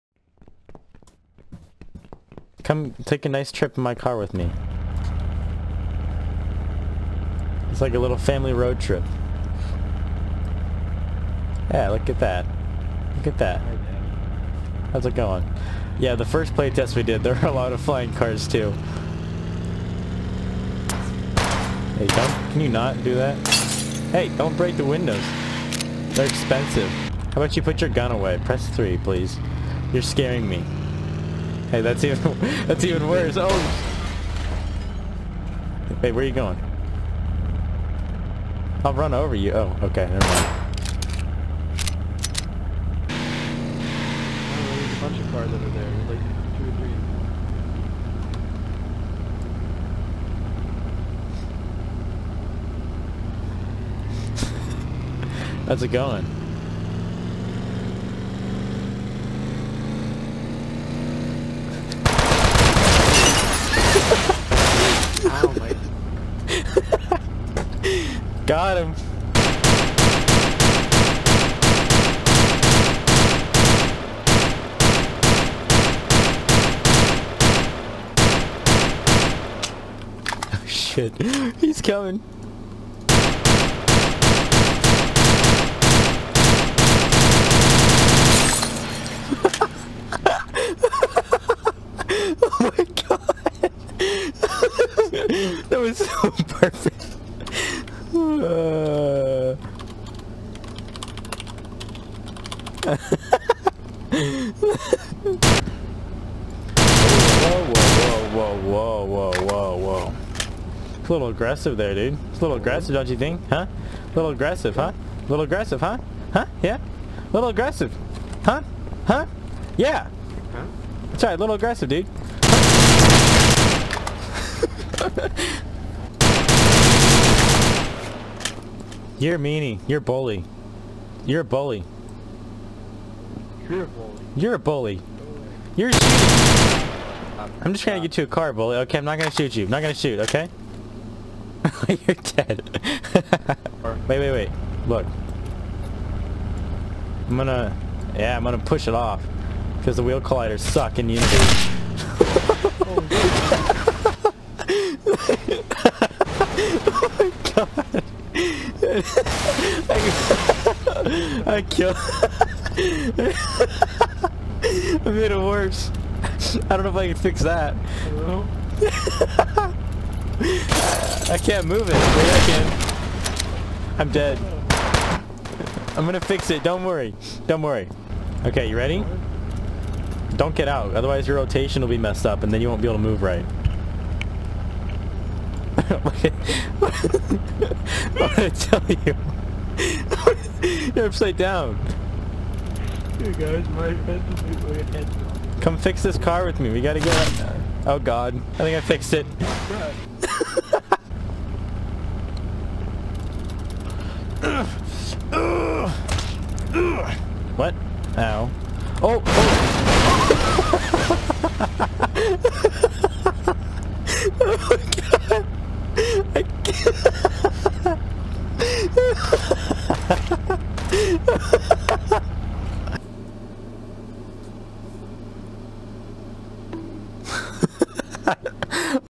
[Unity3D] Open World FPS Prototype (v1)
An experiment heavily inspired by Sub Rosa, Payday 2, and GTA IV & V. The end goal is to have a scenario based 'cops vs robbers' kind of game.